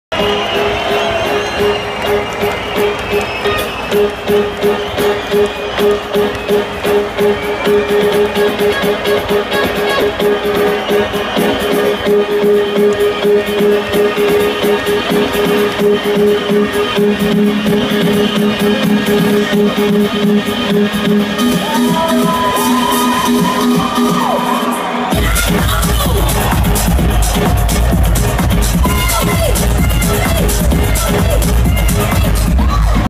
Marco Carola @ Music On Festival Amsterdam 2017 (4)
Marco Carola closing set @ Music On Festival 2017.